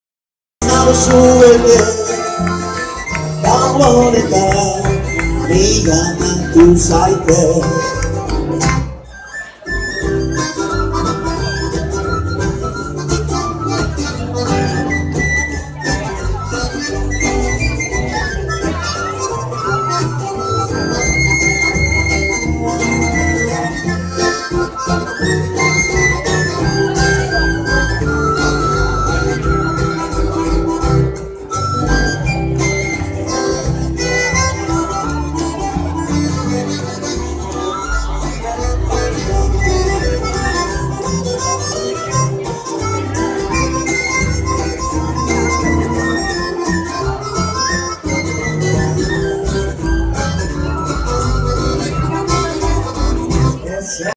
Actuación de Aiko Taldea en lo PIR
Parte de l'actuación de Aiko en lo festival de culturas pirinencas PIR, celebrau lo 7 y 8 de chulio en Chasa (Viello Aragón)